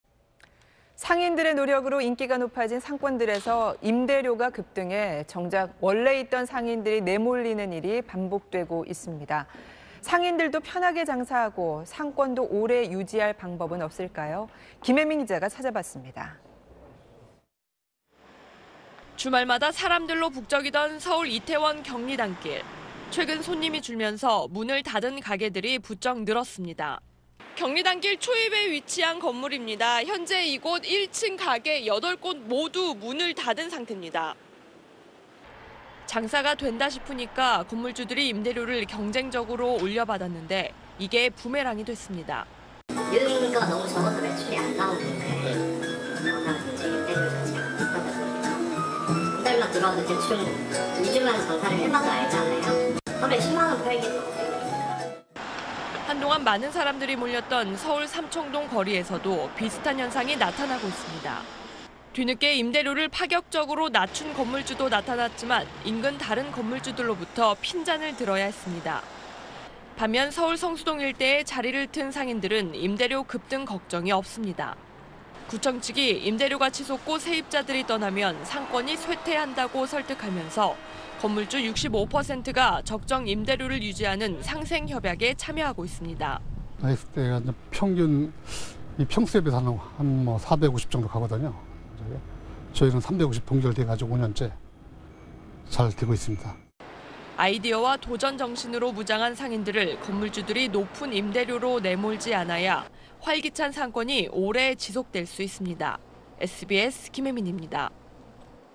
유명세 따라 임대료 천정부지…'핫한 상권' 소리 없는 추락 / SBS
상인들의 노력으로 인기가 높아진 상권들에서 임대료가 급등해 정작 원래 있던 상인들이 내몰리는 일이 반복되고 있습니다. 상인들도 편히 장사하고 상권도 오래 유지할 방법은 없을까요.
김혜민 기자가 찾아봤습니다.